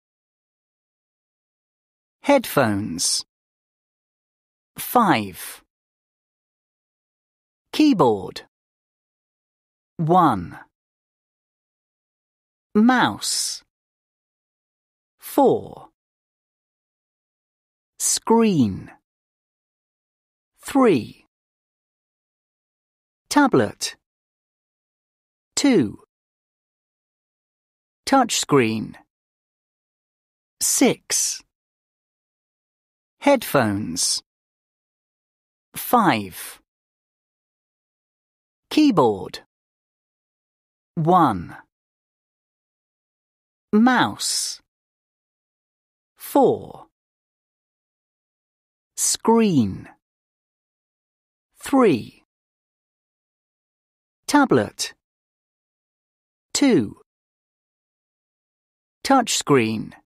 Unit 3 - PB pg. 34 - Activity 3 - Read and say the number. Listen and check.
Vocabulary: headphones, keyboard, mouse, screen, tablet and touchscreen.